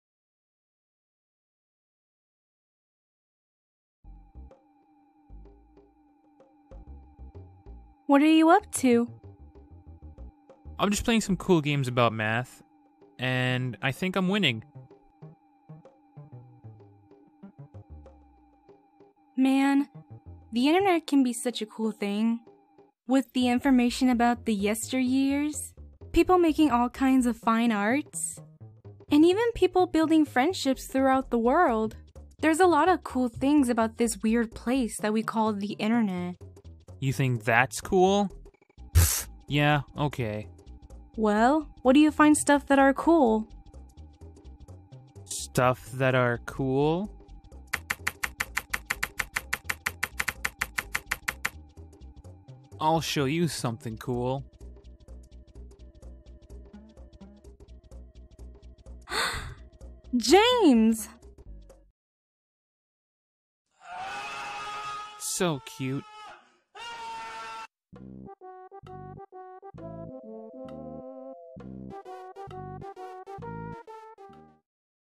THE INTERNET
the internet is kinda cool ig

animation by me! (james)
voices: me and claire